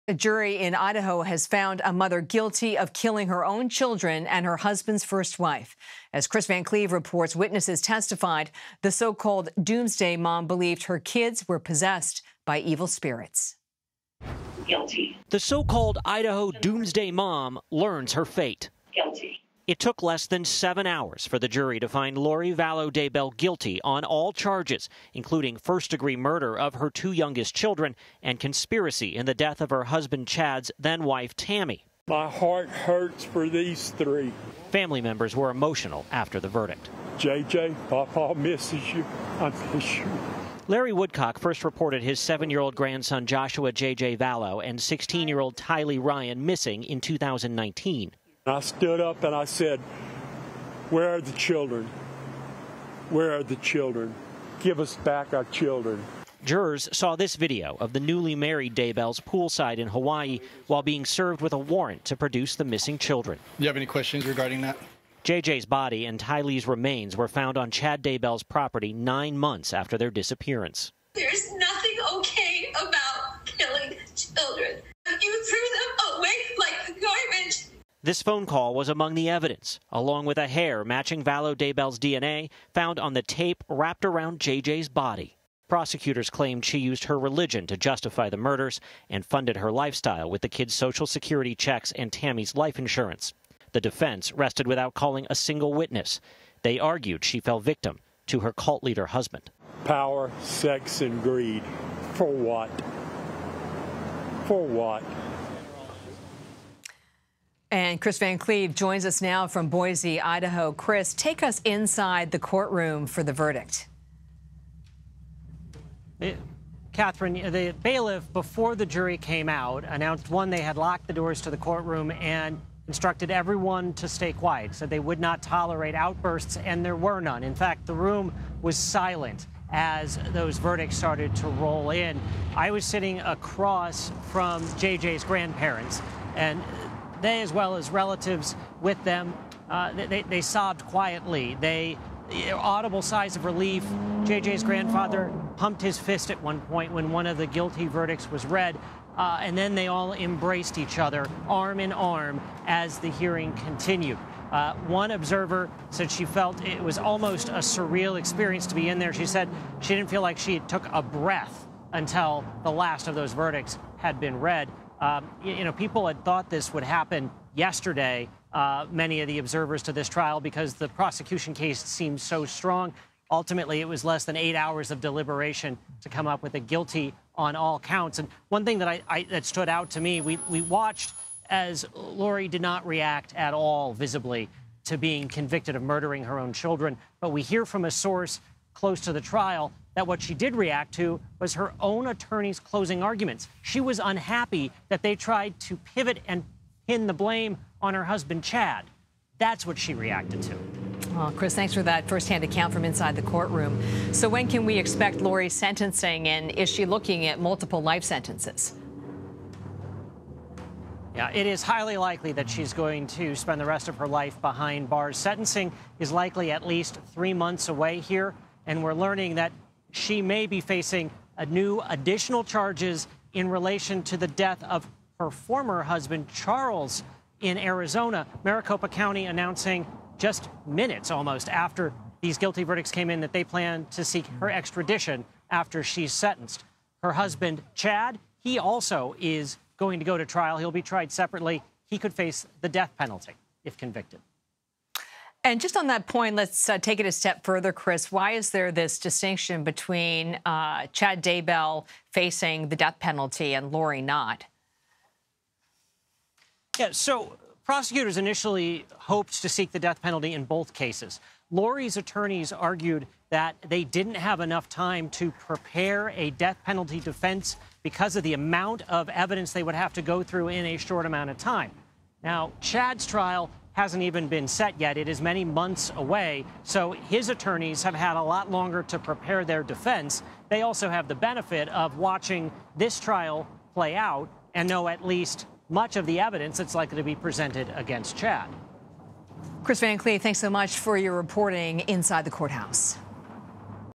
What's next for Lori Vallow Daybell after guilty verdict
A jury in Idaho Friday found a mother guilty of killing two of her children, and found her guilty of conspiring to murder her husband's first wife. Witnesses testified the so-called "doomsday mom" Lori Vallow Daybell believed her kids were possessed by evil spirits. CBS News national correspondent Kris Van Cleave reports from Boise, Idaho.